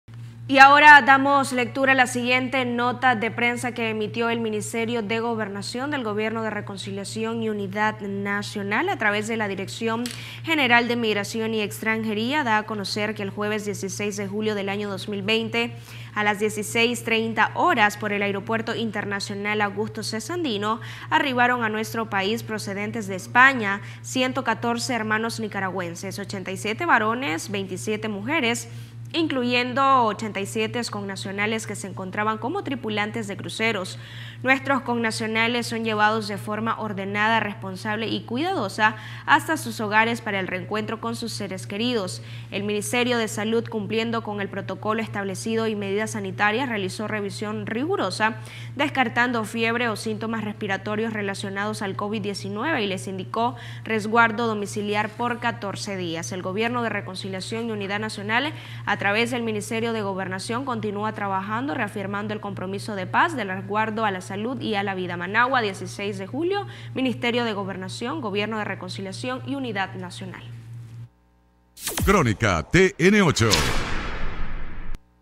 114 nicaragüenses procedentes de España se reencuentran con sus familias - Nicaragua
El Ministerio de Gobernación del Gobierno de Reconciliación y Unidad Nacional, a través de la Dirección General de Migración y Extranjería, da a conocer que el jueves 16 de julio 2020, a las 16:30 horas, por el Aeropuerto Internacional Augusto C. Sandino, arribaron a nuestro país procedentes de España: 114 hermanos nicaragüenses (87 varones, 27 mujeres), incluyendo 87 connacionales que se encontraban como tripulantes de Cruceros.

Te puede interesar: Inicia en Londres la jornada 41/19 "Solidaridad es la ternura de los pueblos"

Nuestros connacionales son llevados de forma ordenada, responsable y cuidadosa, hasta sus hogares para el reencuentro con sus seres queridos.

El Ministerio de Salud, cumpliendo con el Protocolo establecido y medidas sanitarias, realizó revisión rigurosa descartando fiebre o síntomas respiratorios relacionados al Covid -19 y les indicó resguardo domiciliar por 14 días.

El Gobierno de Reconciliación y Unidad Nacional, a través del Ministerio de Gobernación, continúa trabajando, reafirmando el Compromiso de Paz, del Resguardo a la Salud y a la Vida.

Managua, 16 de Julio, 2020
Ministerio de Gobernación
Gobierno de Reconciliación
y Unidad Nacional